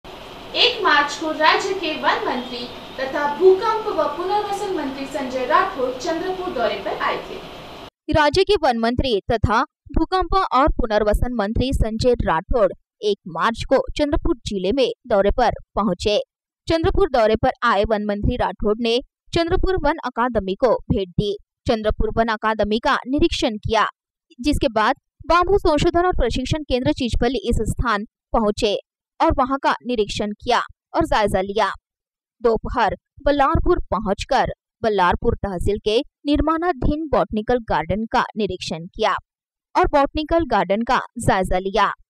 1 मार्च को वनमंत्री संजय राठोड चंद्रपुर दौरे पर City Cable Samachar 01.03.2020
1 मार्च को वनमंत्री संजय राठोड चंद्रपुर दौरे पर City Cable Samachar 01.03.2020